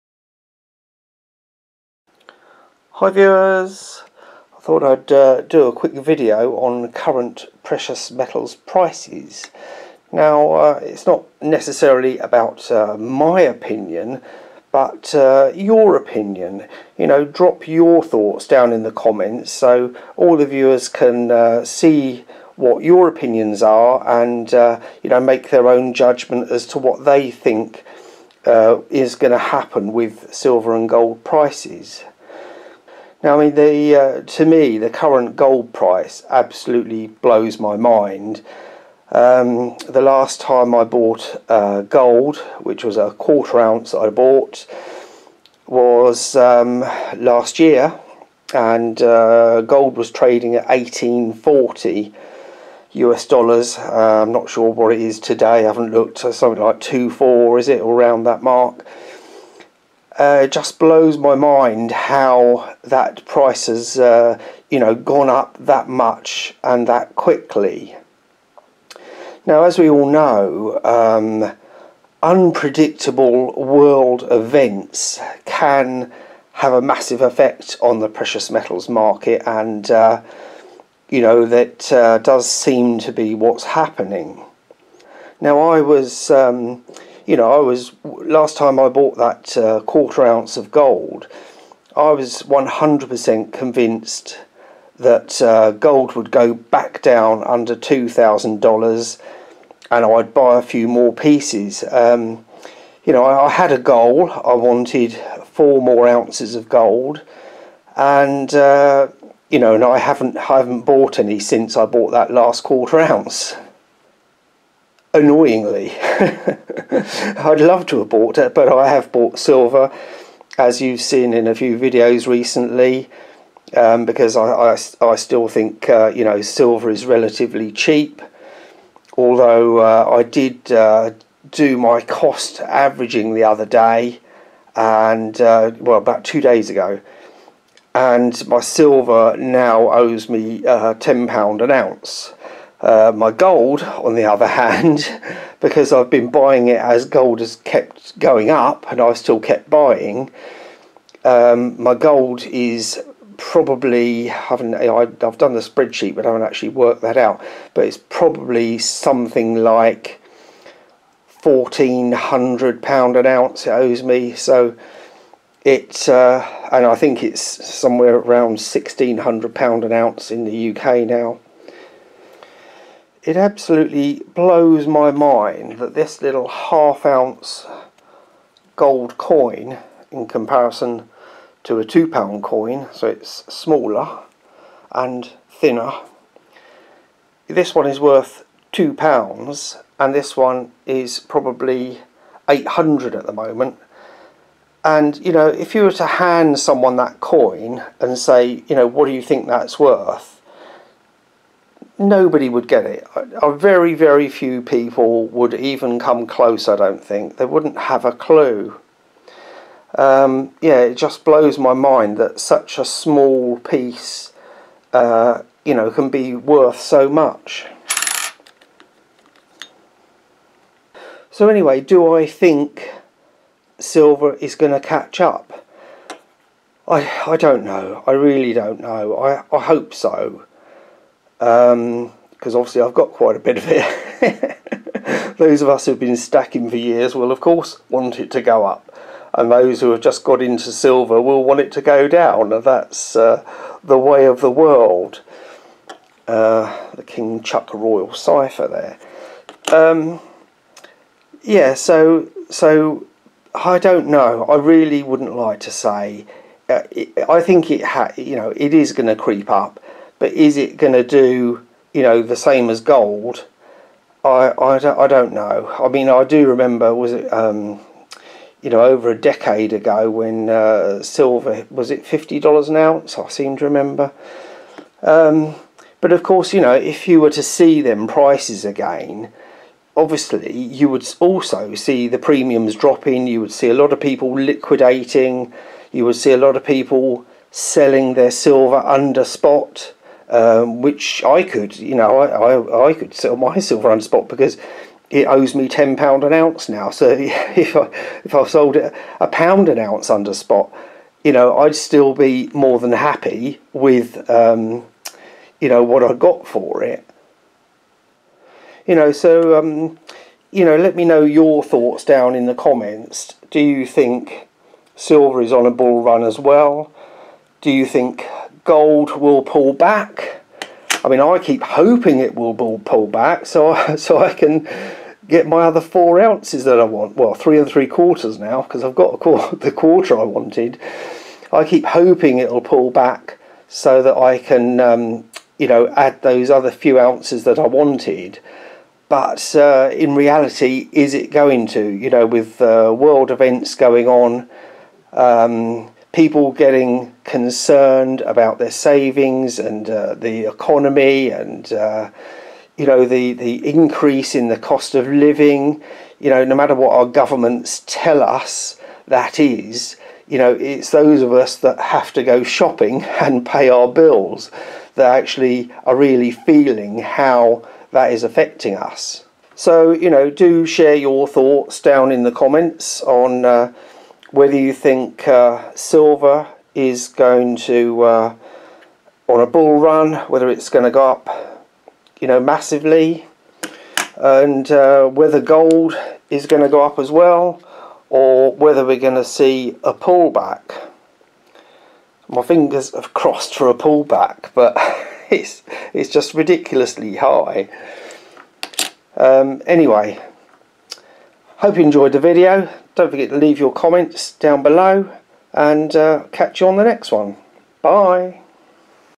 Gold Holding Its All Time Highs. Will Silver Follow Suit?
Let's discuss the current prices of silver and gold & where you think they are headed.